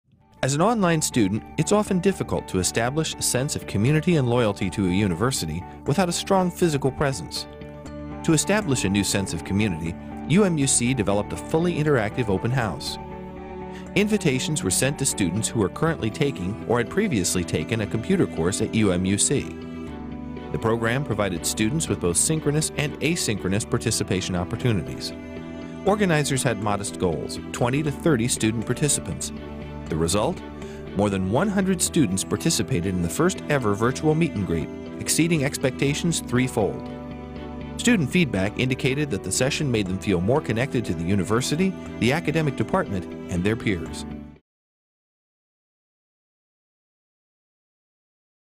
University of Maryland University College: 111 Virtual Meet & Greet
University of Maryland University Colleges UMUC 111 Virtual Meet & Greet for creating a relaxed, online environment for students, faculty and staff to discuss issues of concern to students, to provide help and advice to students, and to create connections that encourage persistence and retention.